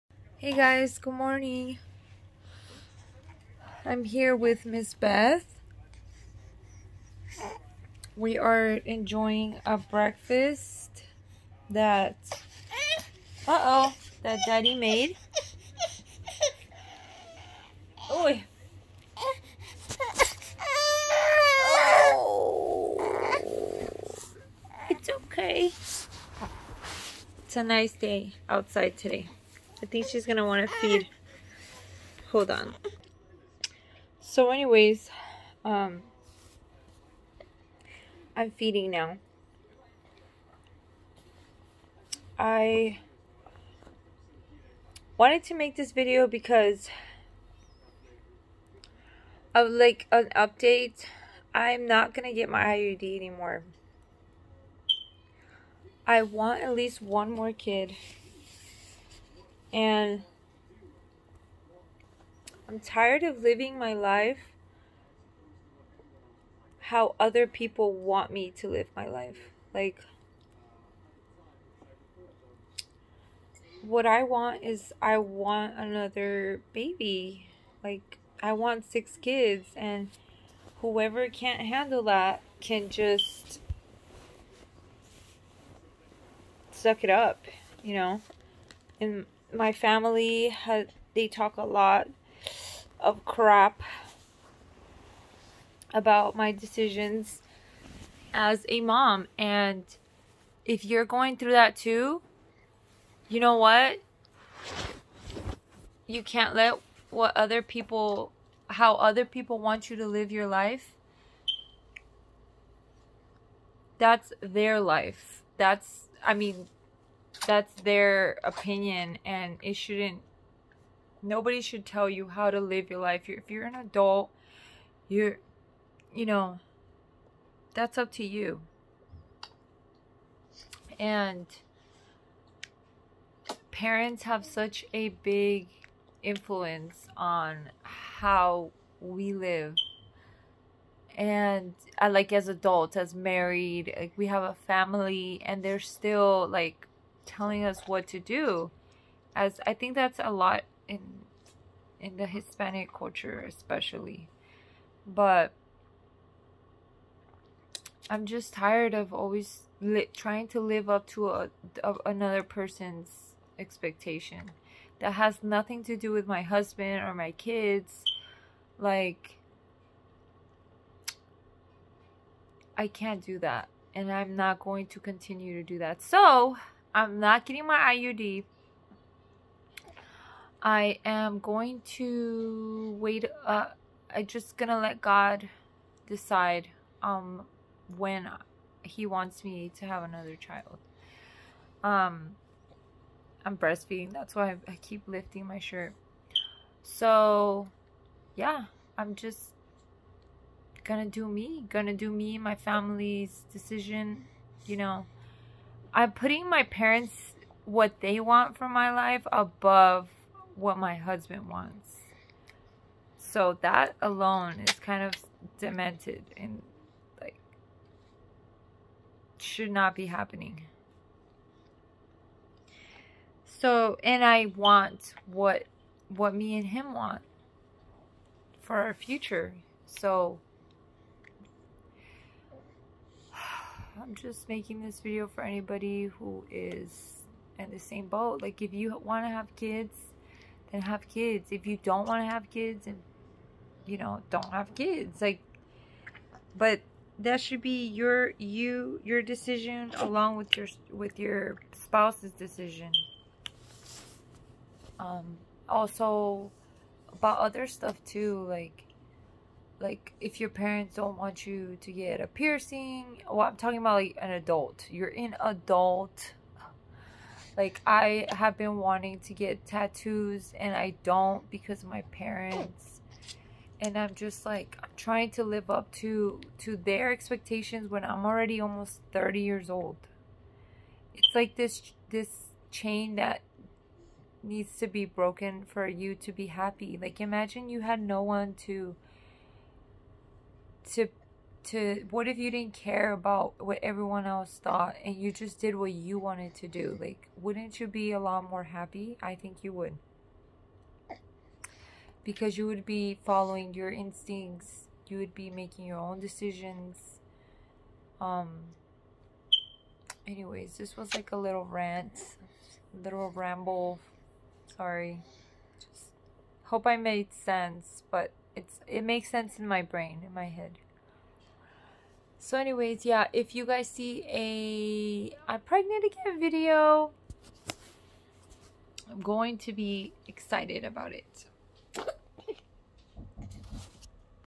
no IUD. MORE KIDS
hey guys I wanted to share what I recently realized and have been trying to change. Godbless